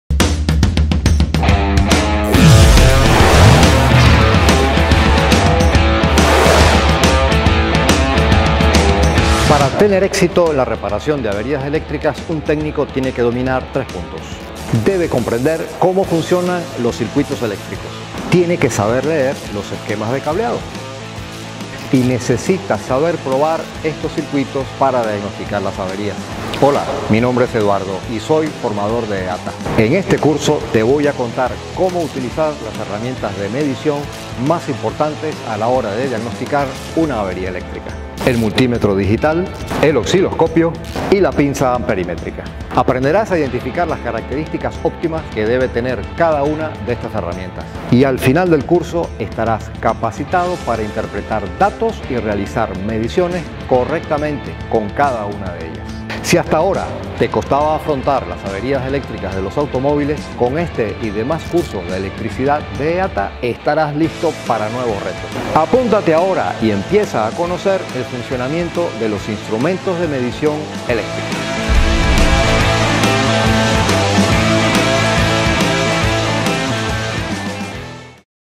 Curso de electricidad nivel 2
Ya tenemos aquí el nivel 2 de nuestro curso de Electricidad, donde aprenderás a seleccionar y a utilizar como un profesional las herramientas principales para la detección de fallas eléctricas en el taller.

Descubre cómo utilizar las herramientas de diagnóstico más habituales, como el multímetro, el osciloscopio y la pinza amperimÉtrica. Interpreta datos y haz mediciones con todas ellas y afronta las averías eléctricas como un profesional.

Aprende todo lo que necesitas sobre la electricidad automotriz en nuestro curso ▶️